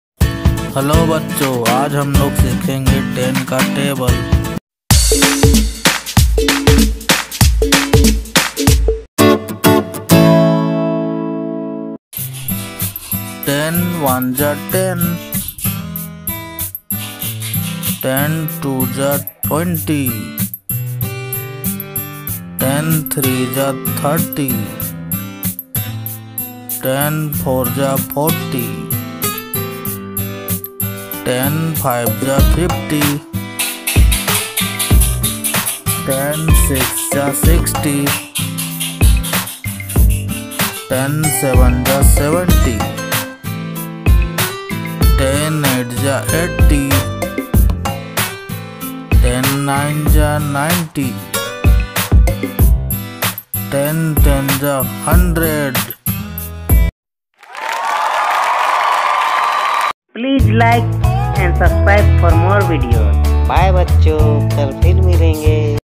Learn Table 10
Hi This is PINTU and your watching PinTooN'S

About this video :
This video is all about learning table 10